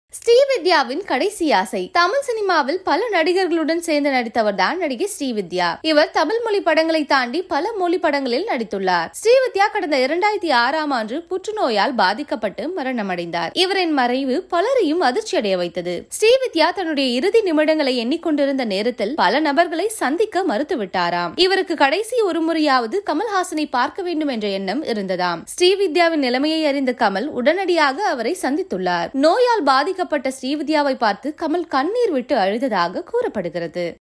ஸ்ரீவித்யாவின் கடைசி ஆசை... | BEHIND GOLD
தமிழ் சினிமாவில் பல நடிகர்களுடன் சேர்ந்து நடித்தவர் தான் நடிகை ஸ்ரீவித்யா. இவர் தமிழ் மொழி படங்களை தாண்டி பல மொழி படங்களில் நடித்துள்ளார்.
ஸ்ரீவித்யா கடந்த 2006 -ம் ஆண்டு புற்று நோயால் பாதிக்கப்பட்டு மரணமடைந்தார். இவரின் மறைவு பலரையும் அதிர்ச்சி அடையவைத்தது.
ஸ்ரீவித்யா தன்னுடைய இறுதி நிமிடங்களை எண்ணிக் கொண்டிருந்த நேரத்தில் பல நபர்களை சந்திக்க மறுத்துவிட்டாராம். இவருக்கு கடைசி ஒரு முறையாவது கமல் ஹாசனை பார்க்க வேண்டும் என்ற எண்ணம் இருந்ததாம்.
ஸ்ரீவித்யாவின் நிலைமையை அறிந்த கமல் உடனடியாக அவர் சந்தித்துள்ளார். நோயால் பாதிக்கப்பட்டு ஸ்ரீவித்யாவை பார்த்து கமல் கண்ணீர் விட்டு அழுததாக கூறப்படுகிறது.